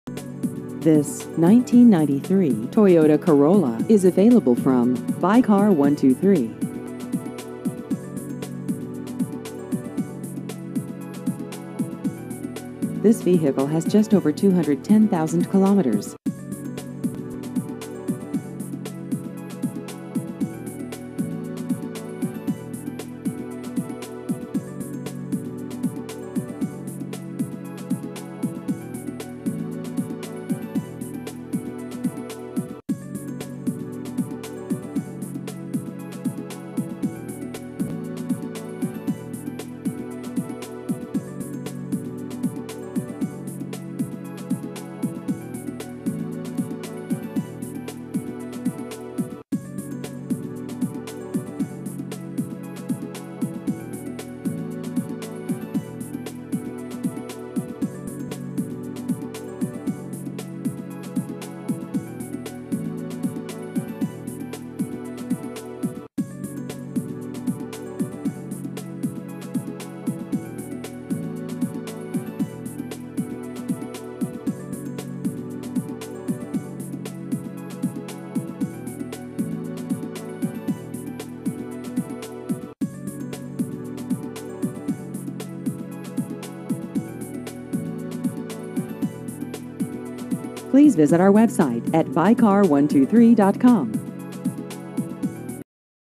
1993 Toyota Corolla Used Cars Vancouver BC
This 1993 Toyota Corolla is available from Buy Car 123.